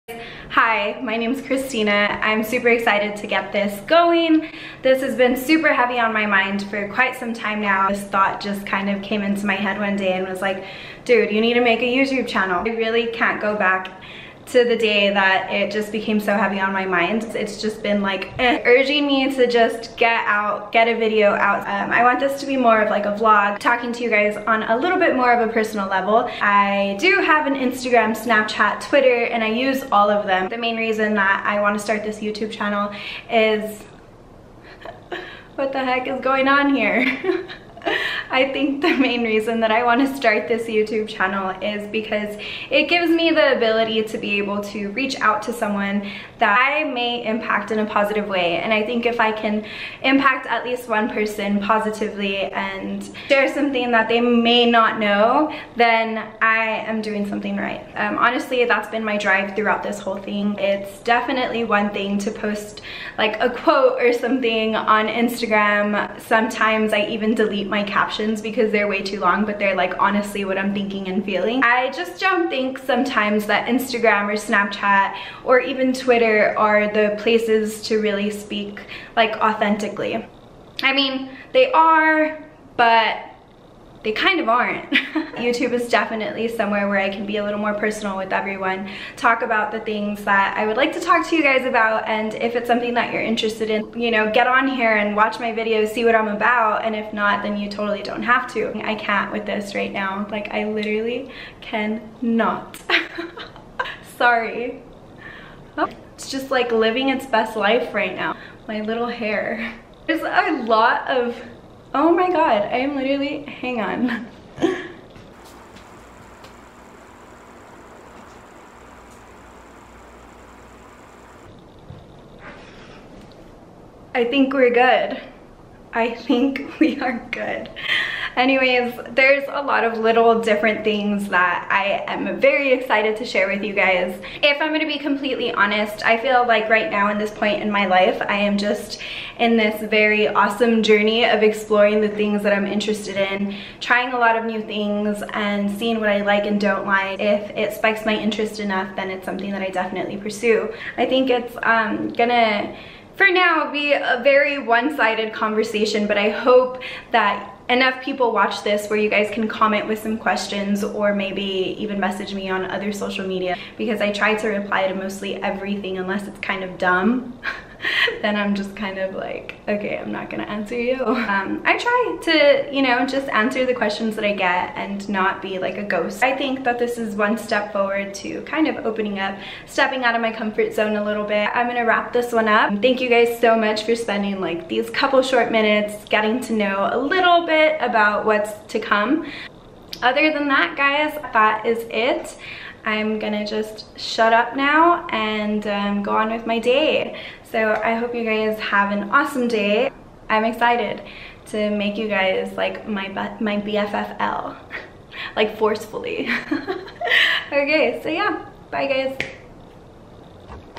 My First Vlog! 🙏♥️✨
Heyyyyy Guys!
Thank you all so much for tuning in, I'm super excited to share my first video with you all! Here it is. Soon I hope to not get so tongue twisted,  and you will be here to see me progress in my whole "talking to a camera" thing.
Talk to you all soon!
xoxo